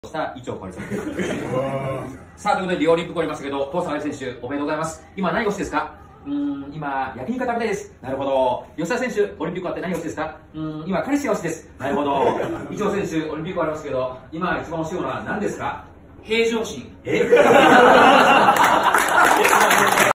オリンピック４連覇した伊調馨さんが、試合後【一番欲しい物】として言った驚きの“物”とは？？
リオデジャネイロオリンピック終わり、インタビューを受ける伊調馨選手が、金メダルを取ったのにも関わらず、自分に厳しいがために言った【今欲しい物】とは？？？

भारतीय फिल्म का ख़तरा